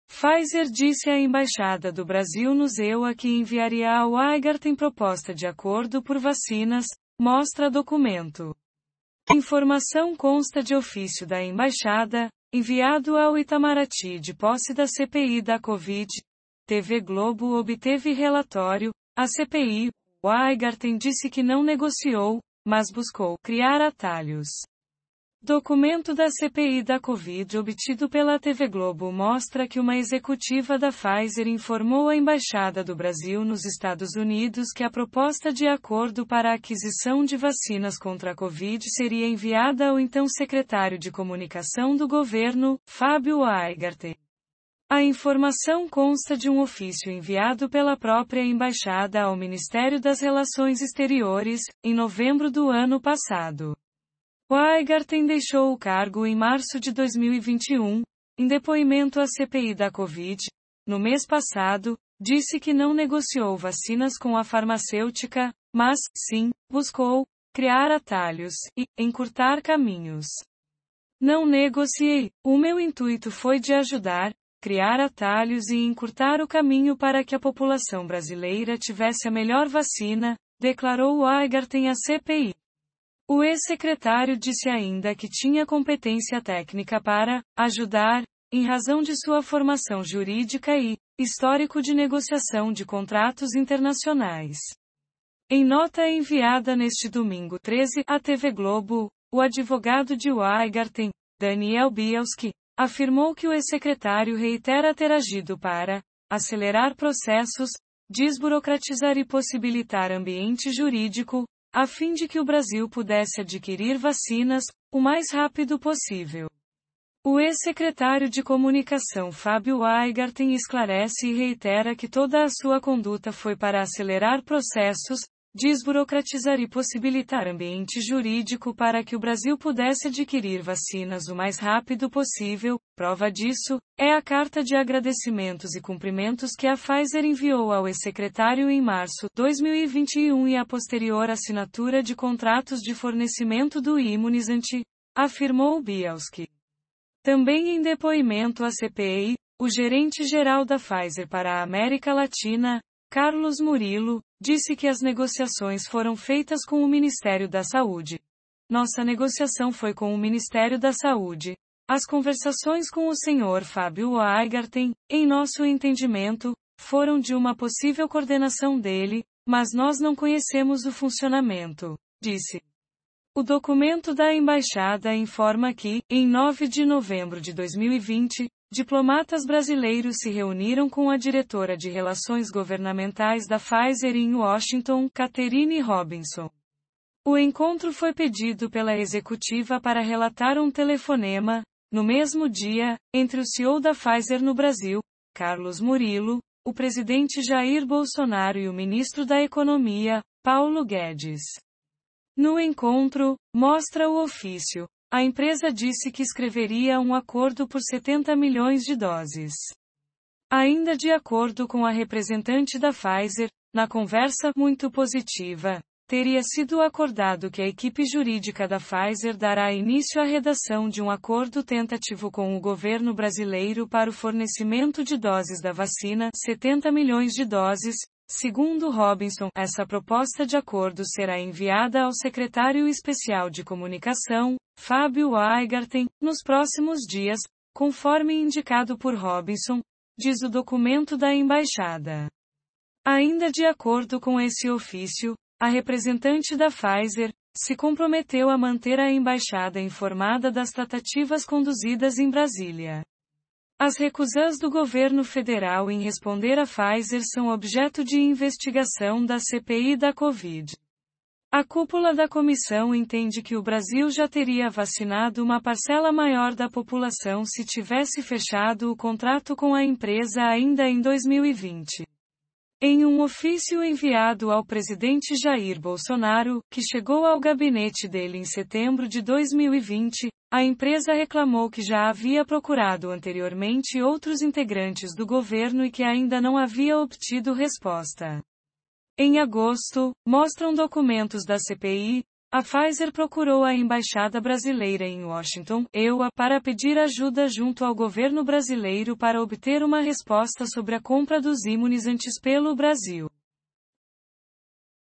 Pfizer disse à embaixada do Brasil nos EUA que enviaria a Wajngarten proposta de acordo por vacinas,
Informação consta de ofício da embaixada, enviado ao Itamaraty e de posse da CPI da Covid; TV Globo obteve relatório. À CPI, Wajngarten disse que não negociou, mas buscou 'criar atalhos'.

Pfizer disse à embaixada do Brasil nos EUA que enviaria a Wajngarten proposta de acordo por vacinas, mostra documento

Acessado em: 14 de junho de 2021 às 06:00